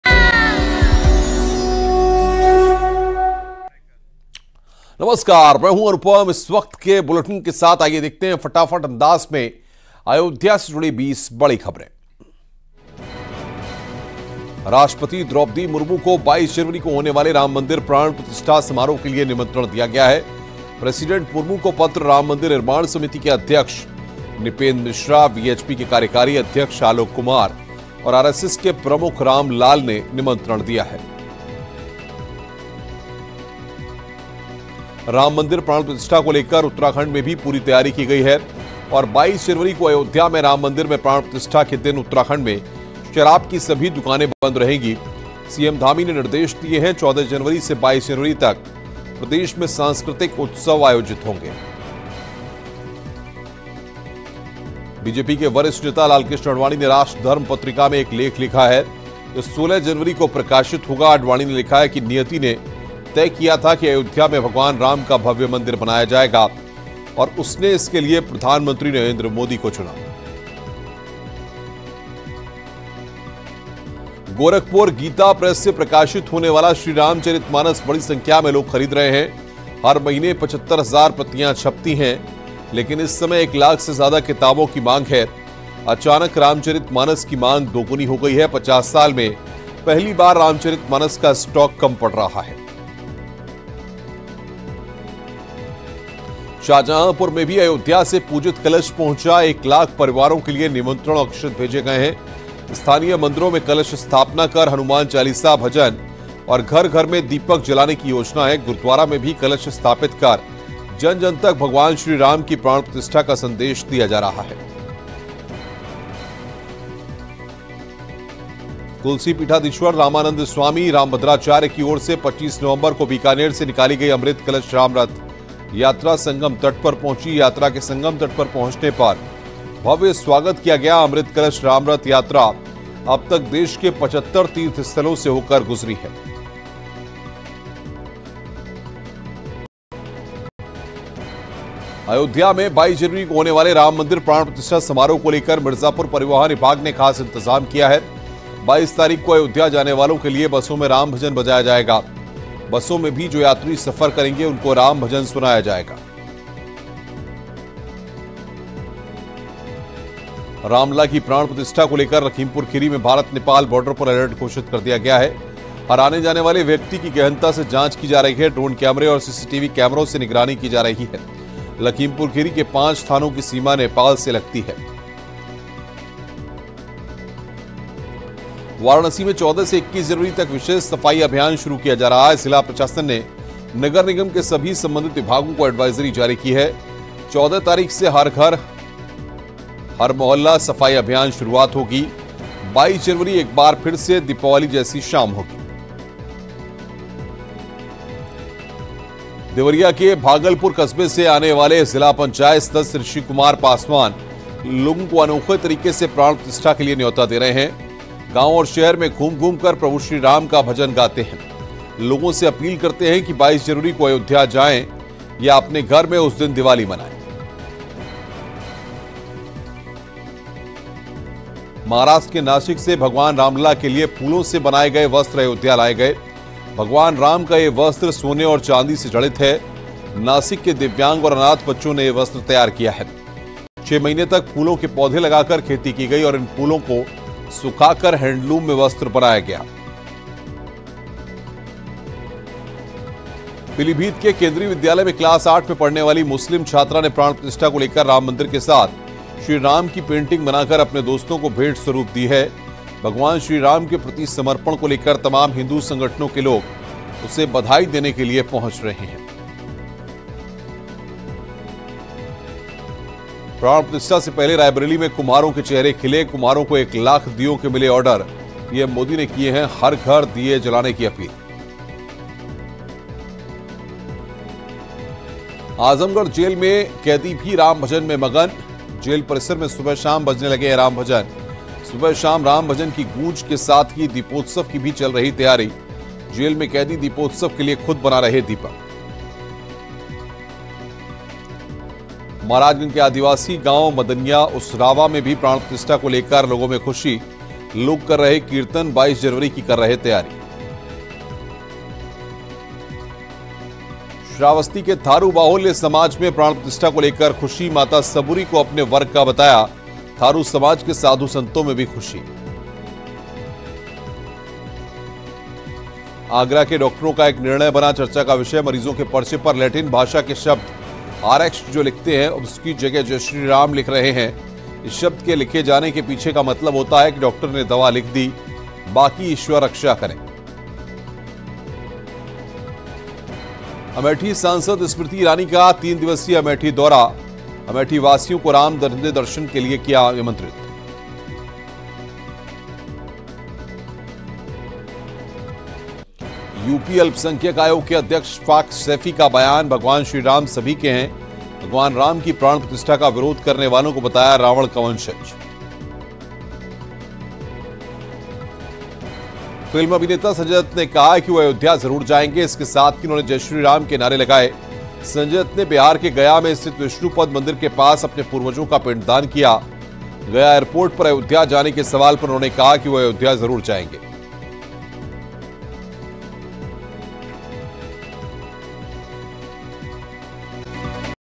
Ayodhya Ram Mandir Top 20 News: अयोध्या जाएंगे Sanjay Dutt। Pran Pratishtha। PM Modi। News 18 UP
राम मंदिर के प्राण प्रतिष्ठा से पहले राष्ट्रपति द्रोपदी मूर्म को निमंत्रण पत्र दिया गया है। वहीं, बॉलीवुड स्टार Sanjay Dutt ने Ayodhya जाने की बात कही है । देखिए पूरी खबर.